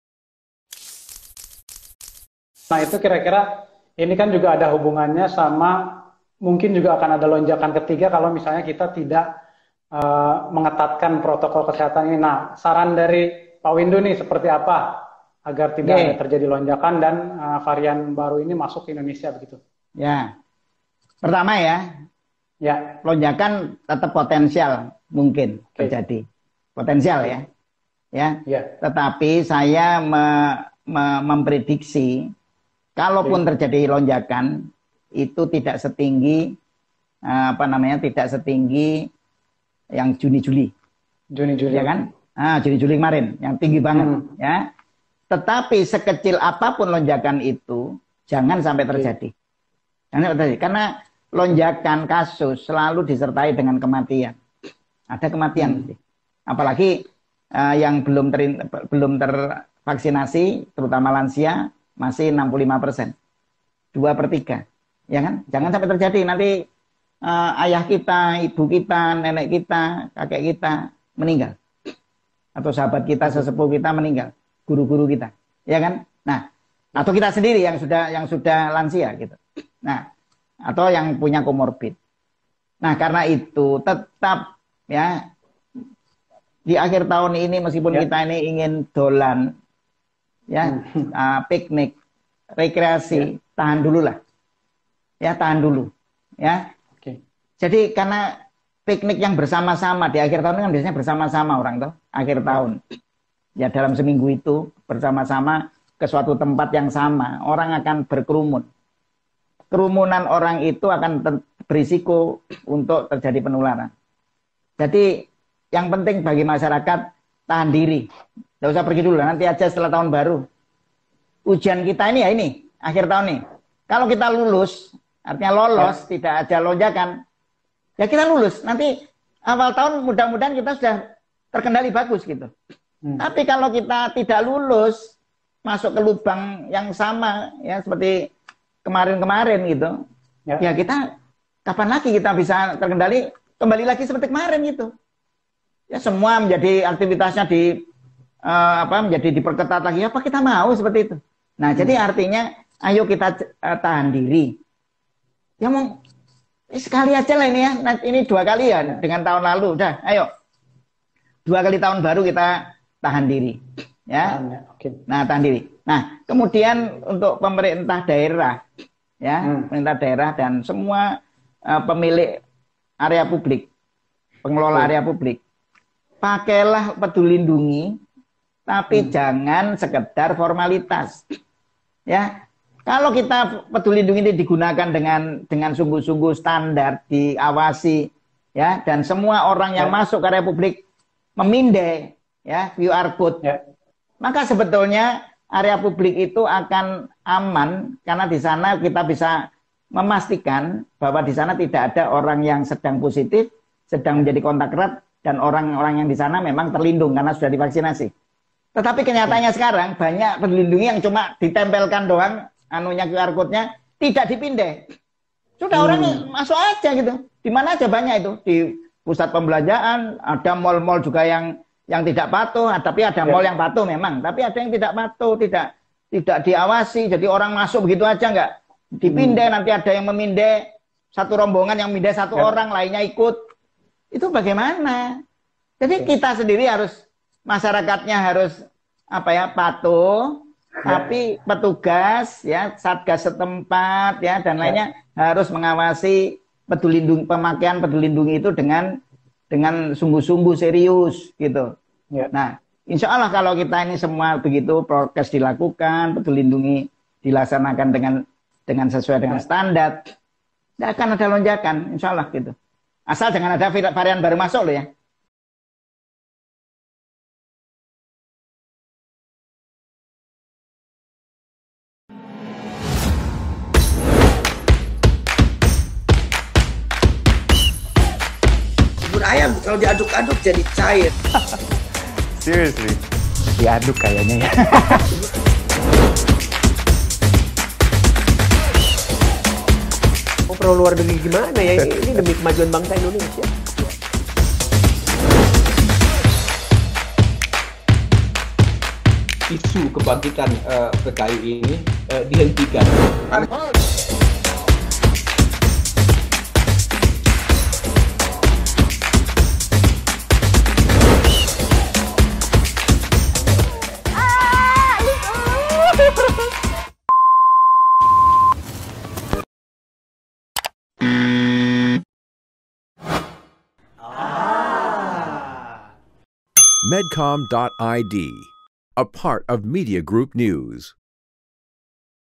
Epidemiolog: Lonjakan Covid-19 Tetap Potensial Terjadi
Epidemiolog Universitas Airlangga Windhu Purnomo mengatakan lonjakan covid-19 tetap potensial. Windhu memprediksi jika terjadi lonjakan tidak akan setinggi sebelumnya namun menurutnya jangan sampai terjadi lonjakan sekecil apapun karena setiap lonjakan disertai dengan kematian. Dengan demikian Windhu meminta masyarakat tetap menjaga protokol kesehatan dan setiap pemilik usaha menggunakan PeduliLindungi dengan benar.